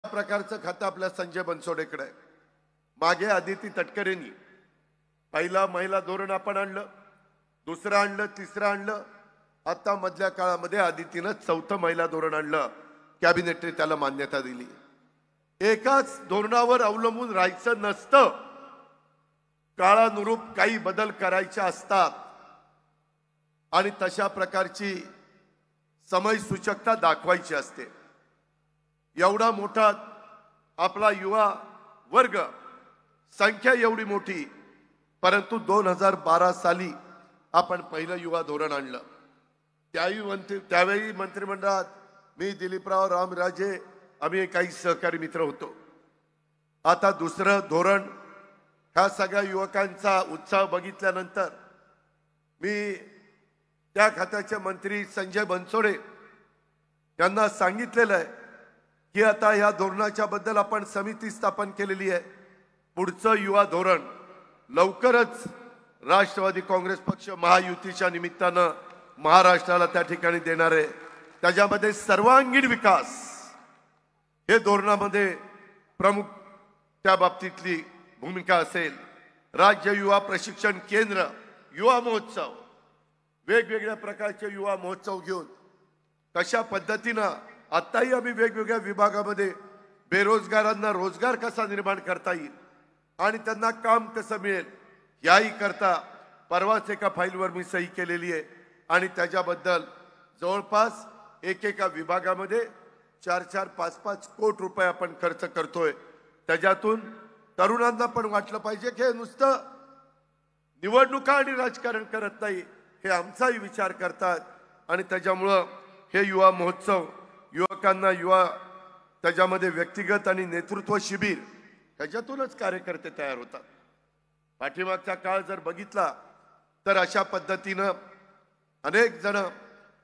Ajit Pawar : एकाच धोरणावर अवलंबून राहायचं नसतं, काळानुरुप बदल करायचे असतात : अजित पवार
Ajit Pawar Dhananjay Munde Pune NCP Melava LIVE | ABP Majha
ABP Majha LIVE TV | Manoj Jarange Patil | Devendra Fadnavis Vs Uddhav Thackeray | Abhishek Ghosalkar